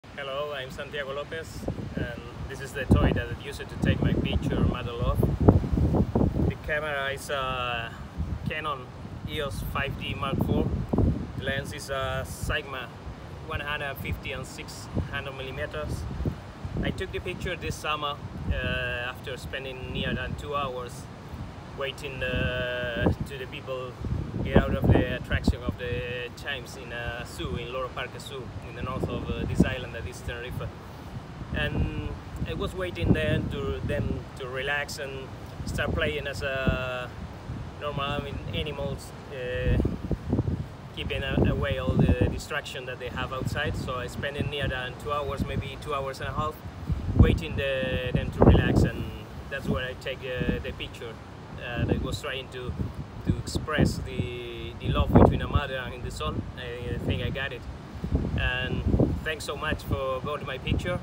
AGORAawards Top 50: Santiago López @santiagolopezfotografia Spain
The world's 50 best photos of the year are out! The AGORAawards2019 contest has lasted 9 months and now it's your turn to vote for the best photo. One photographer will walk away with the grand prize of $25,000 and another with the recognition of the People's Choice Awards. Go to the app and cast your vote! The winner will be announced November 6th 2019.